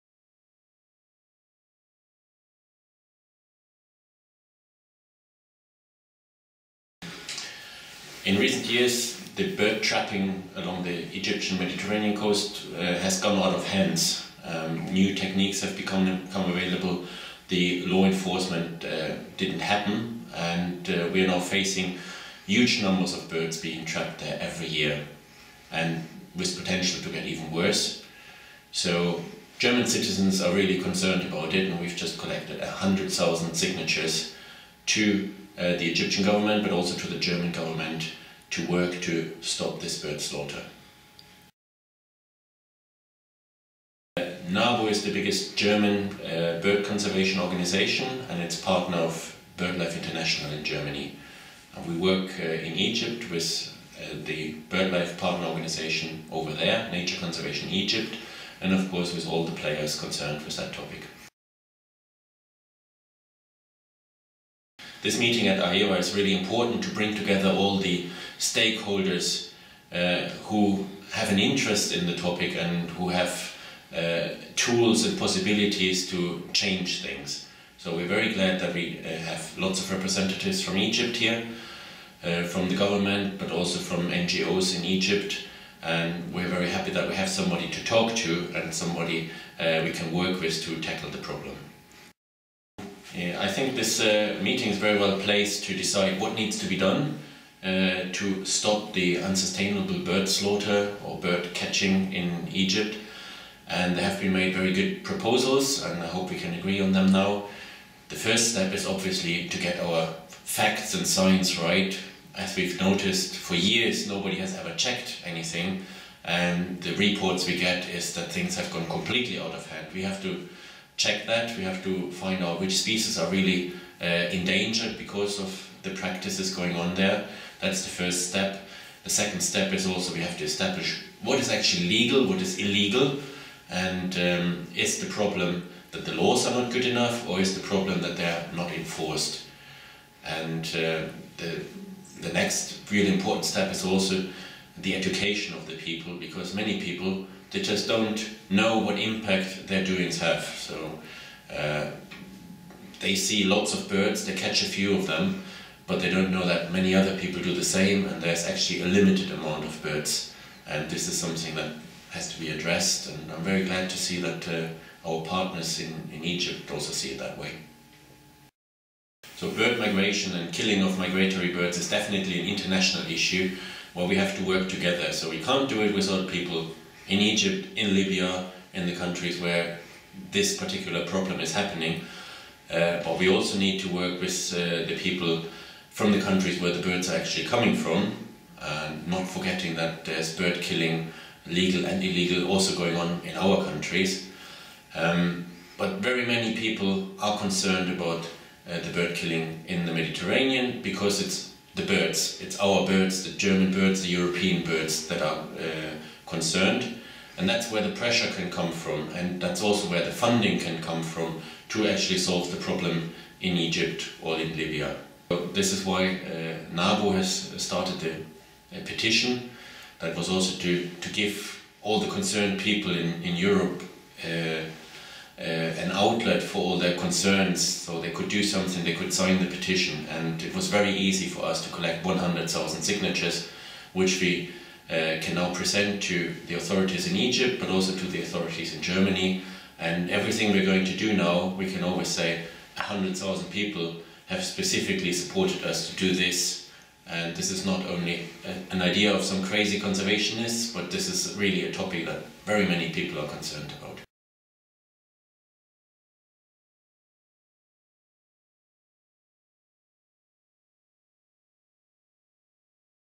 Mr. Lars Lachmann, Bird Conservation Officer, NABU Germany
Interview with Mr. Lars Lachmann, Bird Conservation Officer, Nature and Biodiversity Conservation Union (NABU), during the AEWA Coordination Meeting on Bird Netting in Egypt and Libya, which took place at the UN Premises in Bonn, Germany on 29 November 2013. For more information please visit the AEWA website at http